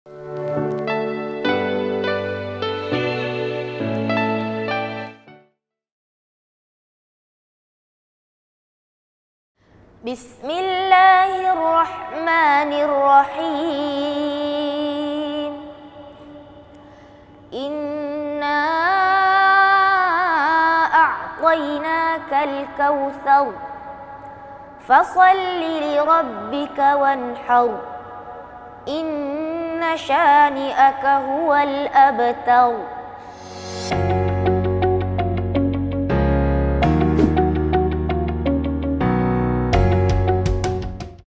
SURAH AL-KAUTHAR - MY TILAWAHTIME
Bacaan Surah Al-Kauthar oleh Muhammad Mikail bin Muhammad Lukman.

Surah Al-Kauthar (سورة الكوثر) adalah Surah Makkiyyah, diturunkan sebelum hijrah Nabi SAW Surah. Surah yang diturunkan selepas turunnya surah Al-A'diyat ini mengandungi 3 ayat dan merupakan surah yang paling pendek dalam Al Quran.

Al-Kauthar bermaksud sebuah sungai yang mengalir di syurga. Surah ini disifatkan sebagai penghibur hati Nabi Muhammad SAW kerana diturunkan ketika baginda bersedih atas kewafatan dua orang yang sangat dikasihinya iaitu anak lelakinya, Ibrahim dan bapa saudara Abu Talib.